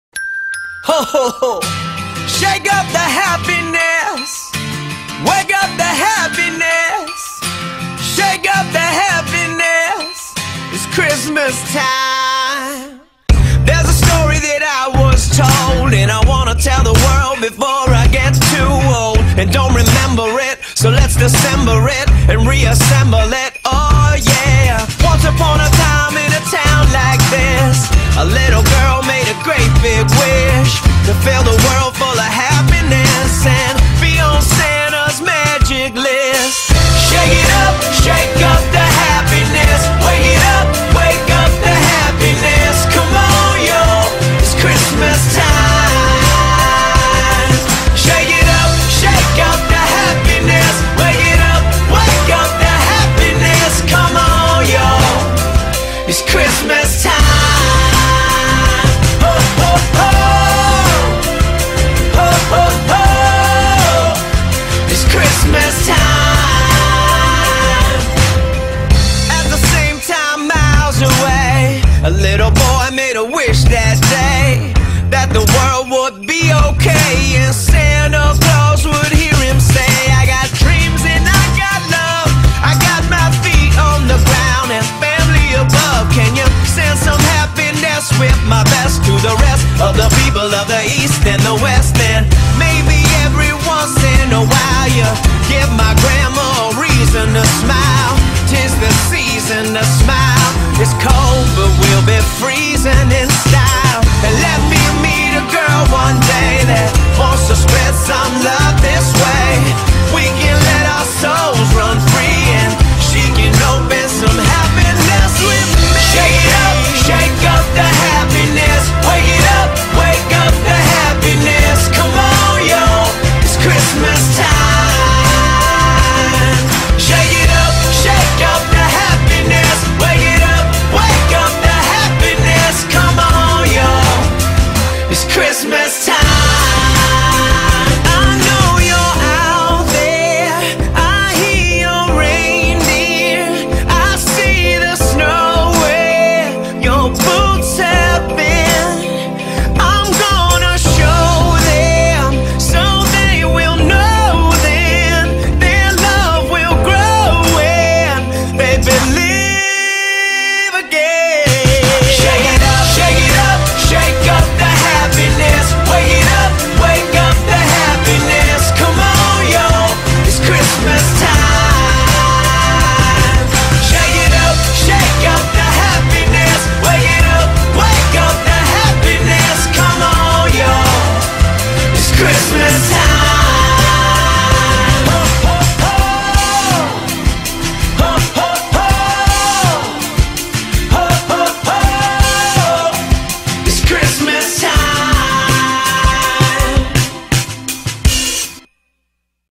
Песочное шоу на Новый год
🎉 Проведение песочного шоу на Новый год Арт-группой SandStorm. Более подробно узнать о шоу песочной анимации можно на сайте